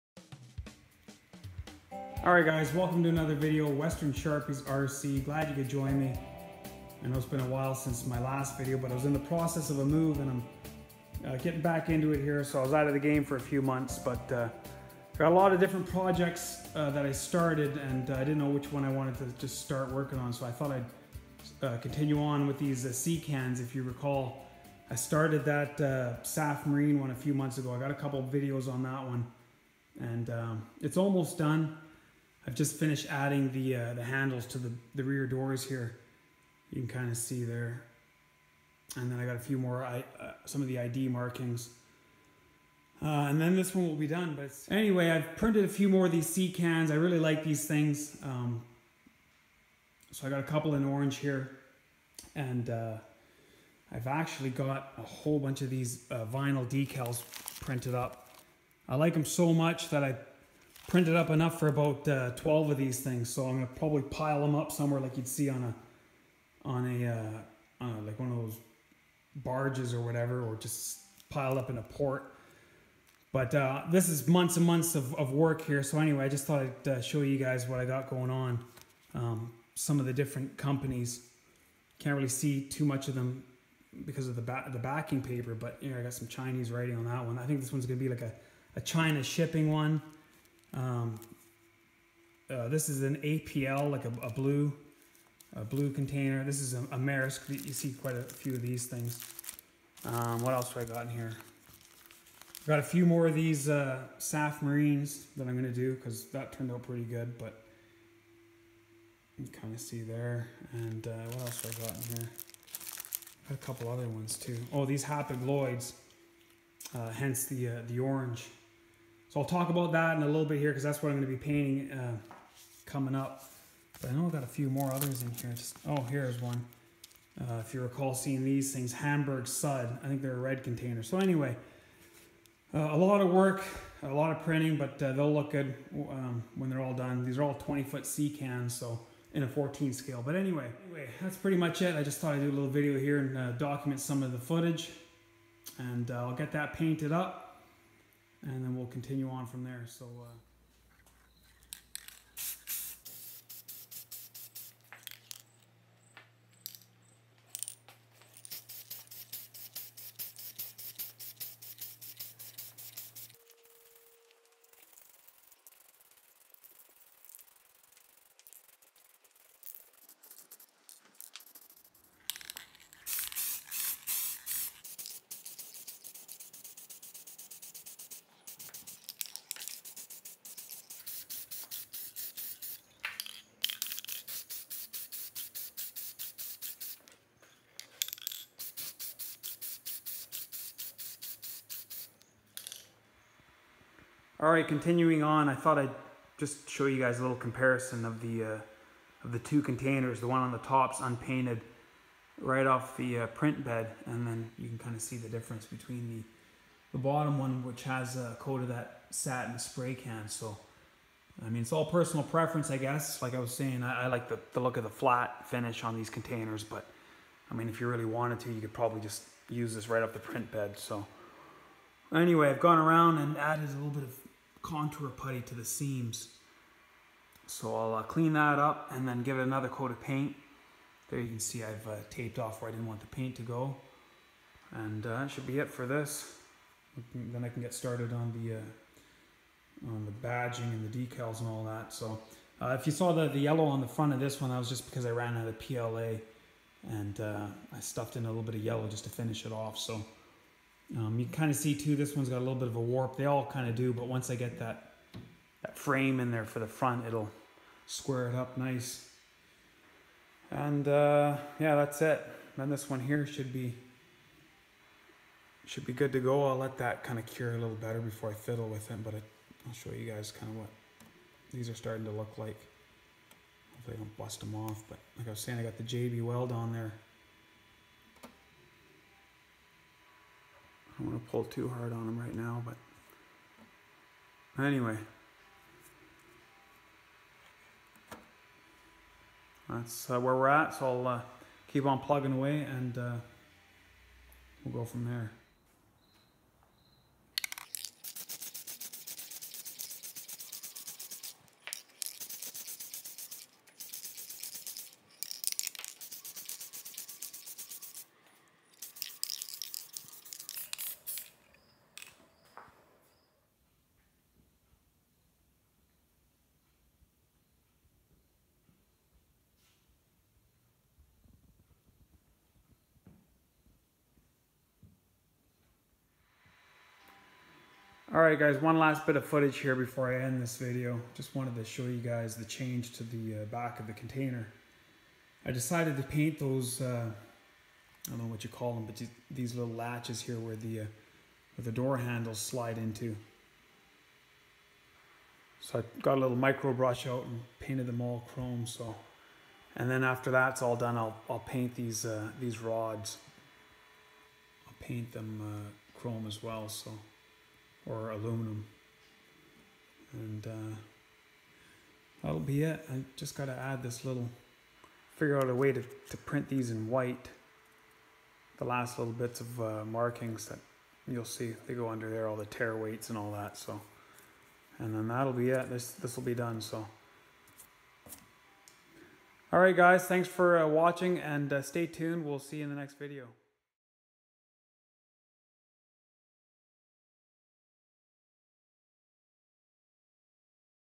1/14 Scale 3d printed 20' sea container. Hapag Lloyd and Saf Marine.
Continuing the work on my 3d printed sea containers. Added a few more bits to my Saf Marine can and started a fresh Hapag Lloyd. Lots more of these to come. Check out my FB store to see some of the scale accessories that are for sale.